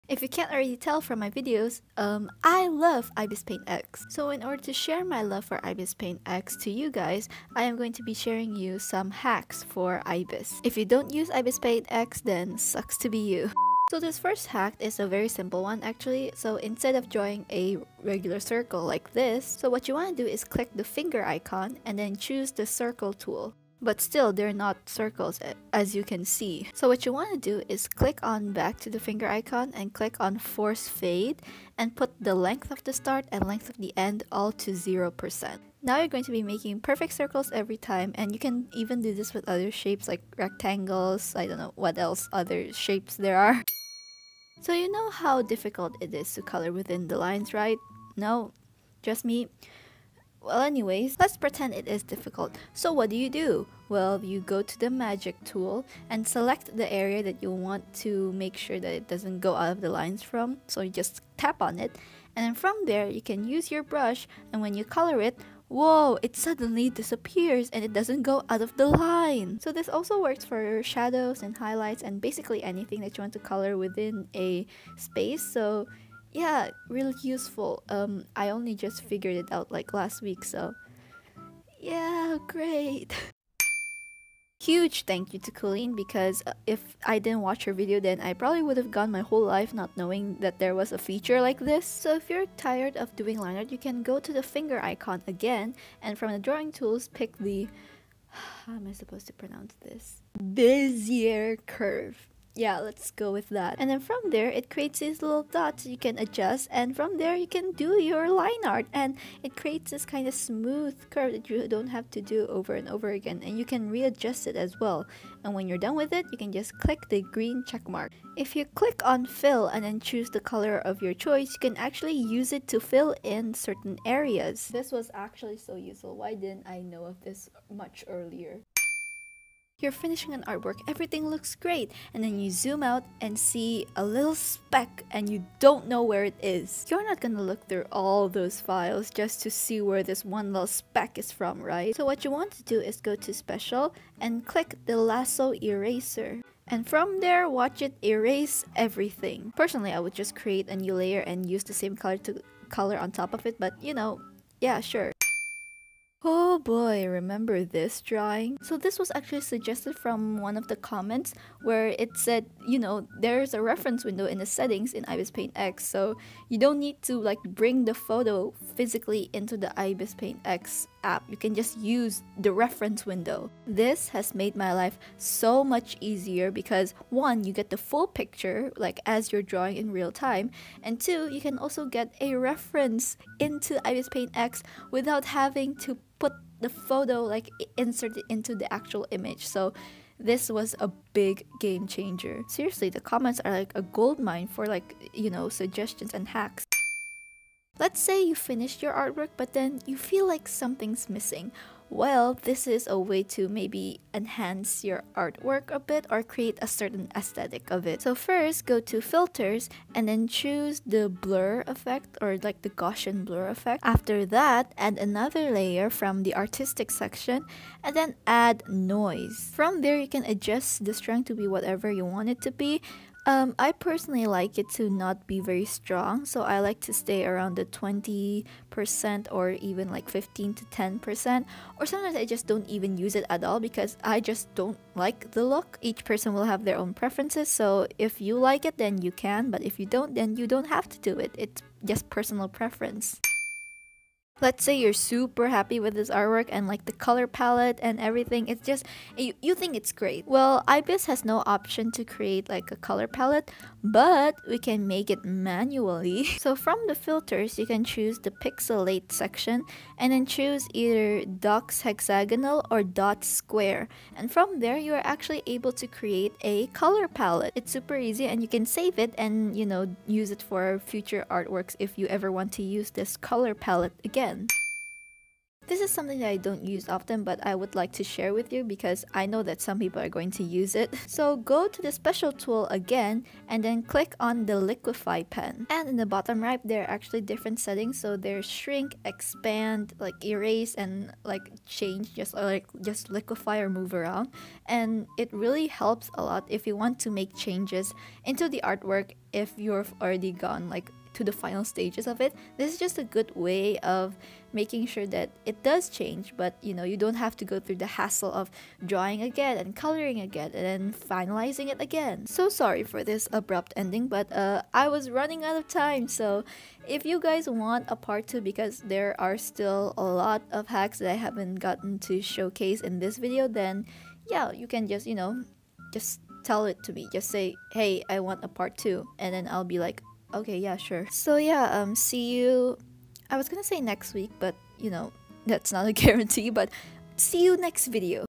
Trying IBISPAINTX HACKS
My love for ibispaintx is slowly growing...
Ibispaintx is supreme
Ibispaintx is amazing
Ibispaintx is wonderful

SWJpc3BhaW50eCBzaGFsbCB0YWtlb3ZlciB0aGUgd29ybGQgYW5kIHdlIHNoYWxsIHJpc2UgYW1vbmcgdGhlIGZsYW1lcw==

0:00 - Intro
0:13 - perfect shapes
0:43 - "colour within the lines"
1:23 - Make line art 1000x easier
2:08 - removing things
2:35 - reference window
3:16 - make it aesthetic
4:03 - Colour palettes
4:34 - every influencer's fav: LIQUIFY
5:12 - Outro